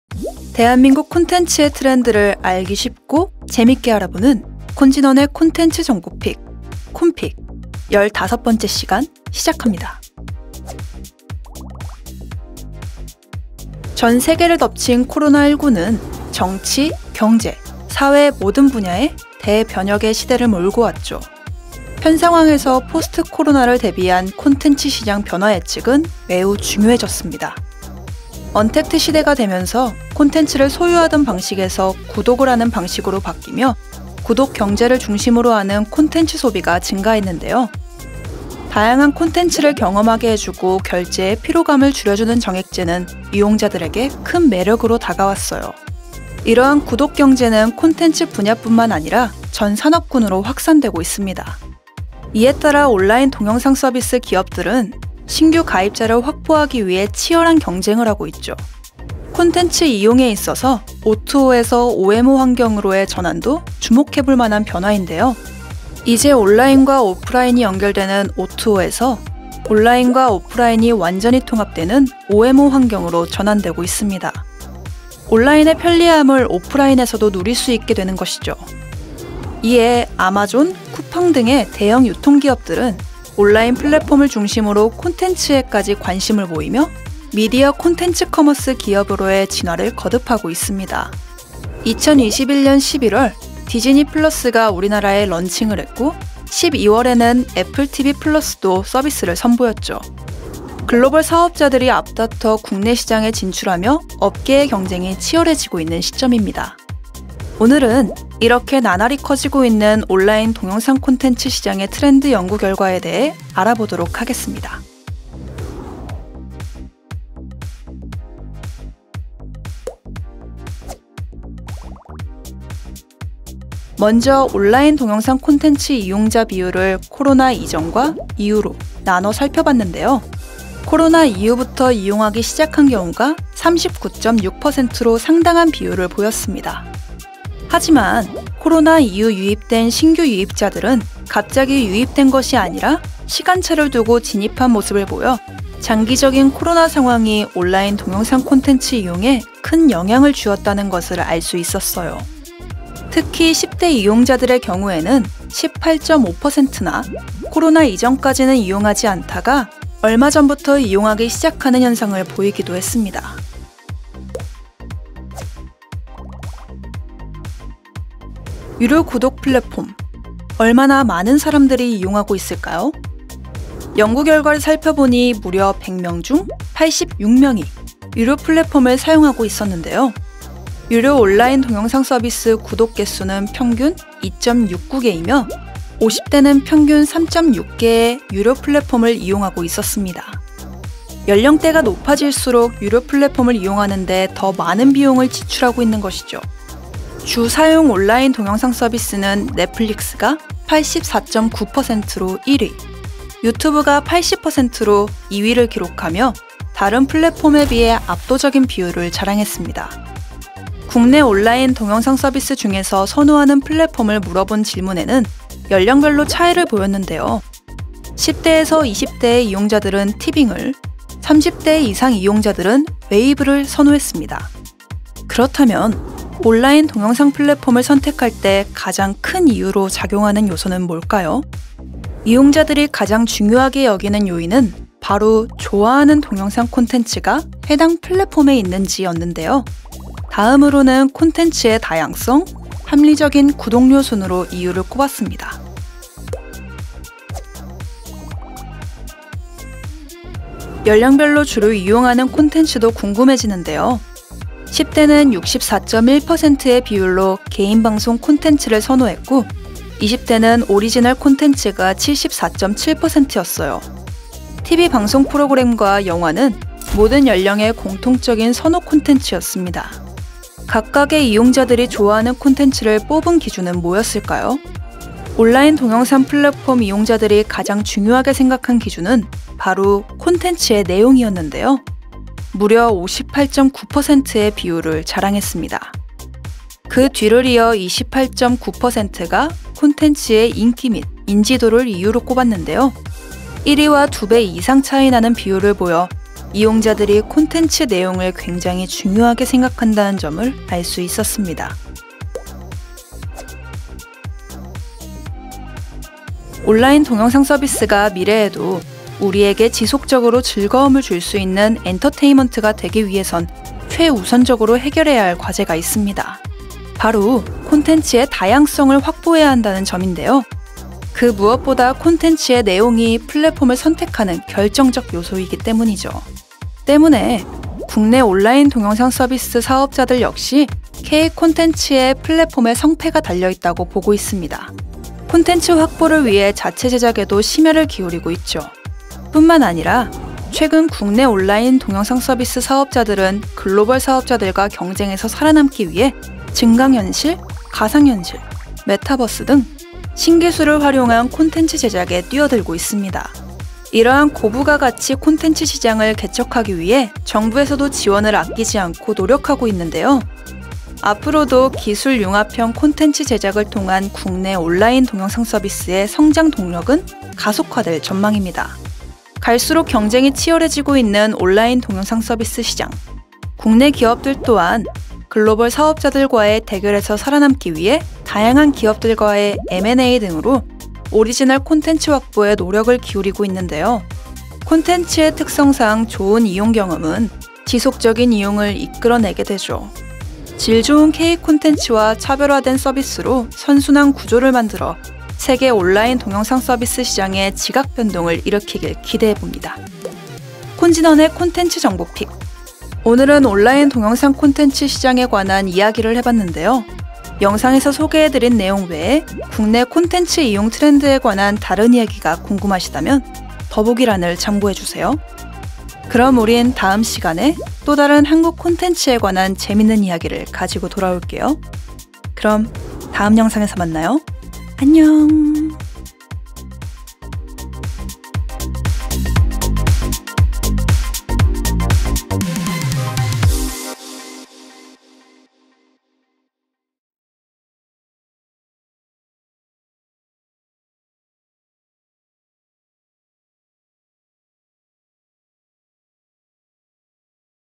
콘텐츠 트렌드 분석, 넷플릭스 OTT 몇프로나 구독할까 한국콘텐츠진흥원 콘픽 Part 15
■ 콘텐츠 트렌드 분석, 넷플릭스와 유튜브프리미엄, 어떤걸 더 많이 구독할까? | 한국콘텐츠진흥원 콘픽 Part.15

코로나는 글로벌적으로 정치, 경제,사회 모든 분야에서 대변혁의 시대를 몰고 왔습니다.
포스트코로나의 시대 콘텐츠 시장 변화 예측이 아주 중요하다는 생각이네요

언텍트시대에는 콘텐츠를 소유하는 시대에서 구독하는 시대로 변경되었고
OTT를 중심으로 넷플릭스 구독 경제를 중심으로 하는 콘텐츠 소비가 증가했습니다.

다양한 콘텐츠를 경험하게 해주며 결제의 피로감을 줄여주는 정액제는 이용자들에게 큰 매력으로 다가왔으며,
구독경제는 콘텐츠 뿐 아니라 전 산업군으로 확산되고 있어요.

콘텐츠 이용도 O2O(오투오)에서 OMO(오엠오) 환경으로 전환이 이루어지고 있어요

■ 참고
O2O (Online to Offline / Offline to Online) - 온라인과 오프라인으로의 전환
OMO (Online mergers with Offline) - 온라인과 오프라인의 완전한 통합

온라인의 편리함을 오프라인에서도 누릴 수 있게 되었다는~~~:)

아마존의 프라임, 쿠팡플레이 등 대형 플랫폼 기업들은 미디어 콘텐츠 커머스 기업으로 진화하고 있습니다.

2021년 11월 디즈니플러스 한국 런칭,
2021년 12월 애플TV플러스 서비스 시작.

이렇듯 온라인 동영상 콘텐츠 시장의 트렌드 연구 결과에 대해 알아보겠습니다 :)

콘텐츠 트렌드 분석, 계속 영상을 보시려면 01:55 이후부터 확인해주세요^^

● Chapter

00:00 intro
00:12 데이터경제,구독경제,초개인화,OMO
01:56 2021 디지털전환시대 콘텐츠 이용 트렌드 연구 보고서 참조
02:00 온라인 통영상 콘텐츠 이용
02:46 지불 및 구독
04:09 주이용 콘텐츠 및 선호
05:05 온라인 동영상 서비스 전망
06:40 콘진원 콘텐츠정보 Pick
07:13 다음 영상
07:27 finish